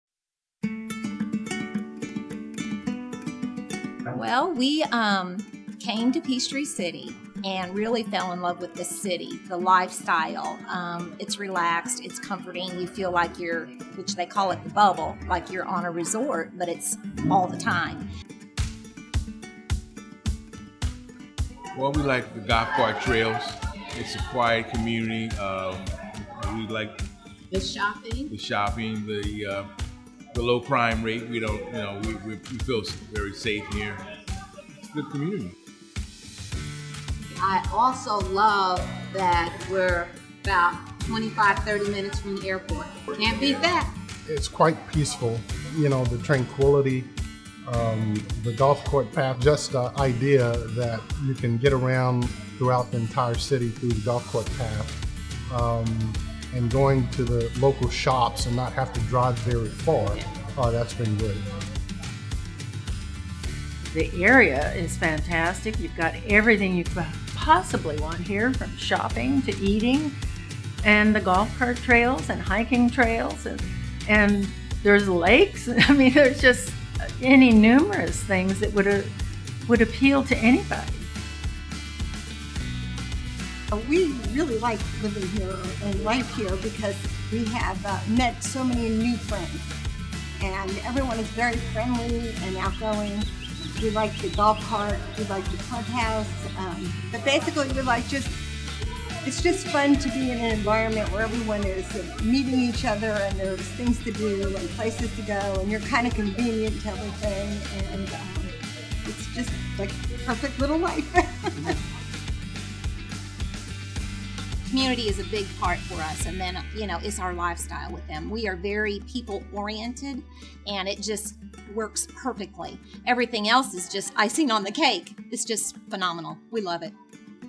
Cresswind Peachtree City: Residents' Perspective
Hear what residents have to say about their new home community's location and lifestyle. Cresswind Peachtree City by Kolter Homes was voted the Atlanta-area's #1 55+ Community by the GAHBA.